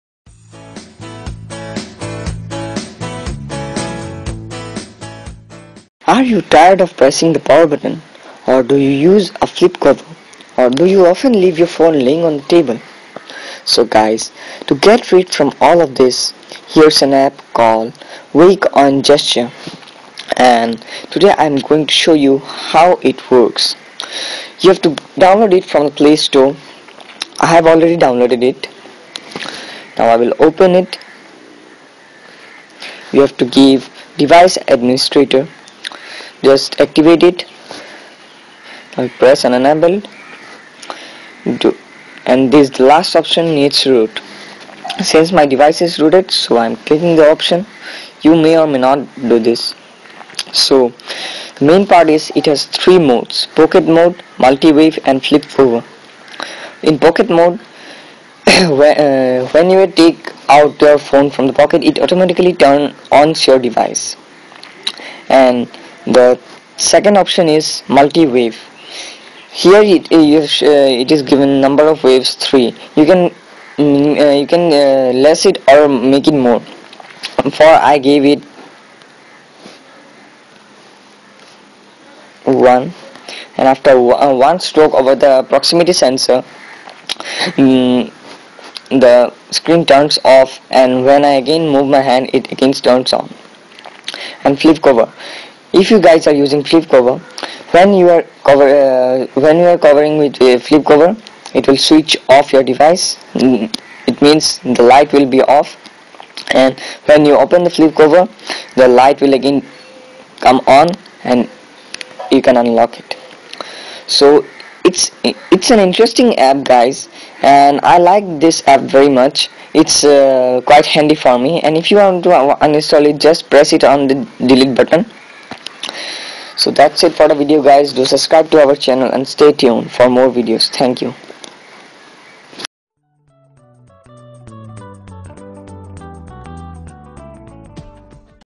Wake on gesture - android app
IN THIS VIDEO I HAVE SHOWN YOU HOW TO  WAKE UP YOUR DEVICE ON GESTURE!!